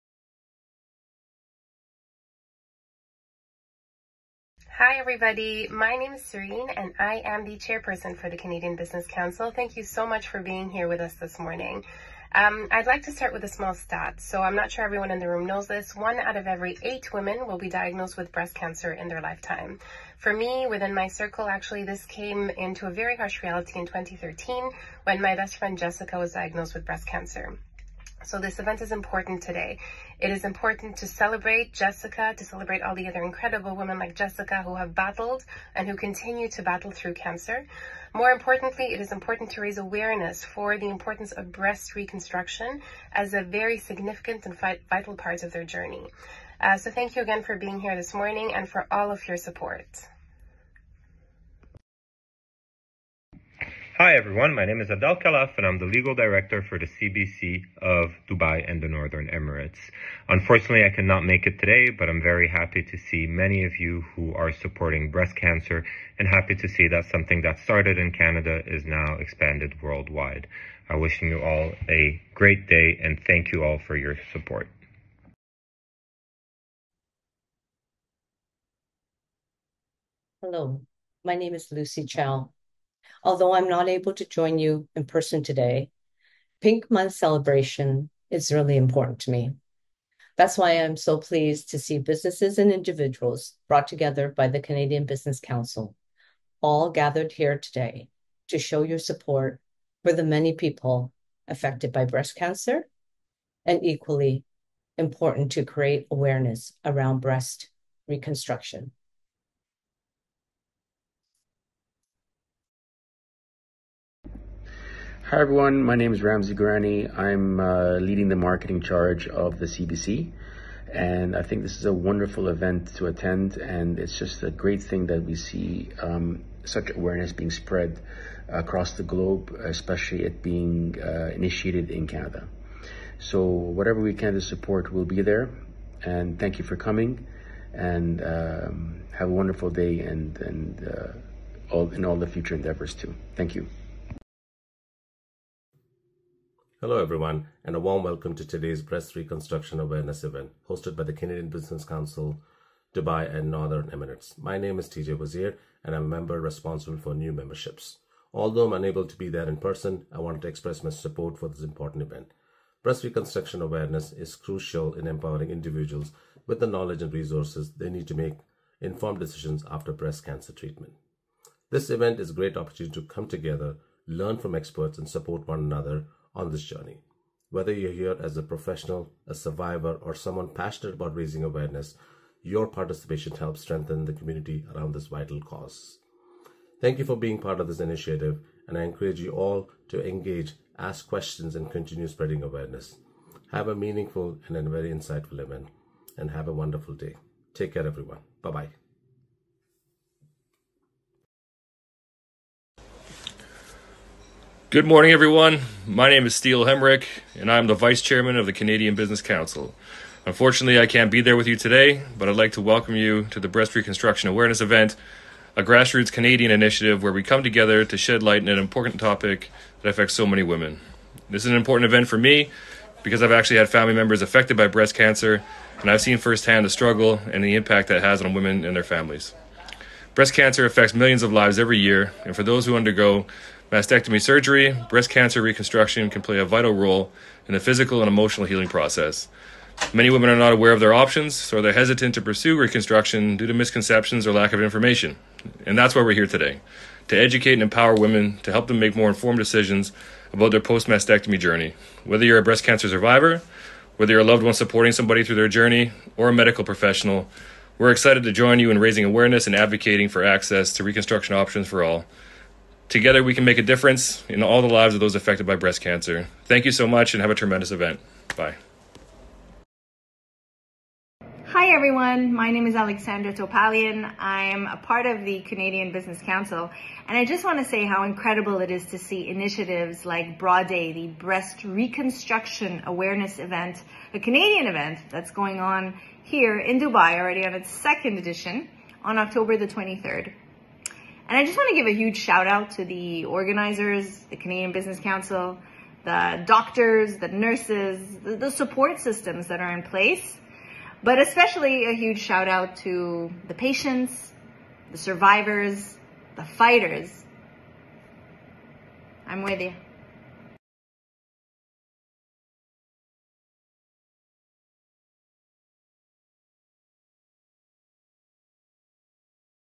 CBC Dubai BRA Day- Breast Reconstruction Awareness Day 2024 Welcome Message
Join us for an inspiring event: BRA Day - UAE 2nd Edition!

As we come together in the spirit of Pink Month, we warmly invite you to join us for a special morning dedicated to raising awareness for breast reconstruction. This event is a testament to the strength and resilience of patients, families, and healthcare professionals who continue to support each other on the path to healing.

📅 Date: Wednesday, 23 October 2024
⏰ Time: 08:30 – 10:30
📍 Location: DoubleTree by Hilton Business Bay, Dubai

We are honored to host Dr. Stefano Pompei  renowned reconstructive plastic surgeon, and Dr. Serife Simsek breast surgeon in Dubai. This celebration is not only about spreading knowledge but also about the power of community and care.

A heartfelt thank you to the Canadian Business Council of Dubai and the Northern Emirates’ Executive Team, our stakeholders, Turkish Business Council in Dubai & Northern Emirates, Hilton  and everyone who has contributed to making this event a success. Your unwavering support is invaluable. We encourage you to share this event and watch the short video clip below to join us in this meaningful cause.

Let’s unite in raising awareness and supporting those impacted by breast cancer.
RSVP today and be part of this important initiative!

Mucha Hazel Nyandoro
Sereen Hindawi
Steele Hemmerich
Ramzi Ghurani    Cert. CII
Lucy Chow
Adel Khalaf
Alexandra Topalian
Wazeer "TJ" Jalal
Diana Szyszka-Pompei
Sangita Bhatia